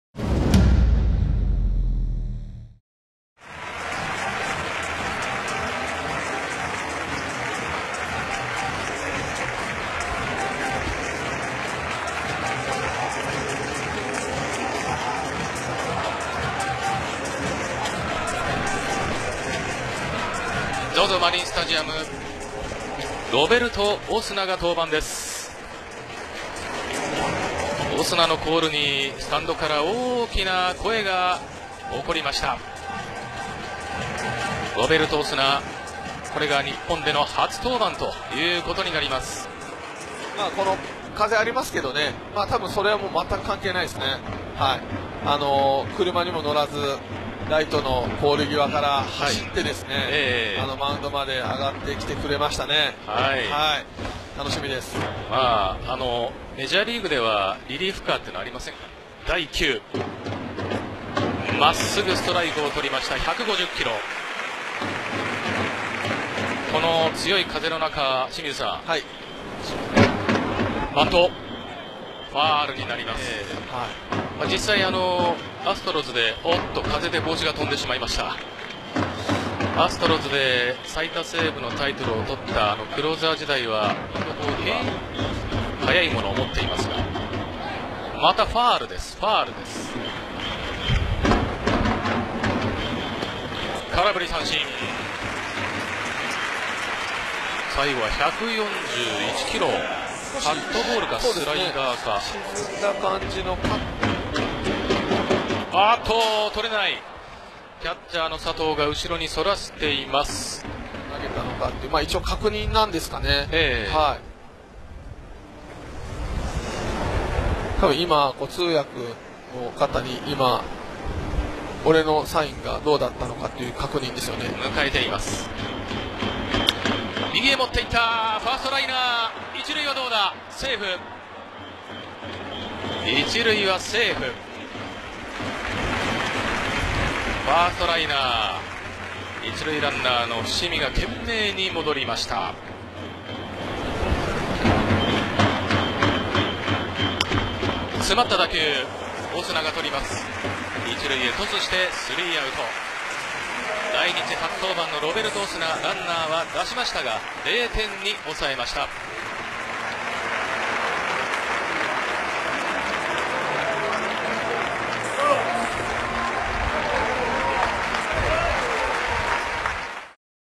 Roberto Osuna has breezy Japan debut | Foreign Exposure 06/24/22
Chiba Lotte reliever Roberto Osuna, a recent addition to the team, makes his Japan debut on June 24, tossing a scoreless inning in a 5-0 loss to the Orix Buffaloes at Zozo Marine Stadium. Osuna, who loses his cap at one point in the windy stadium, walks one and allows a hit in putting up a zero.

Roberto Osuna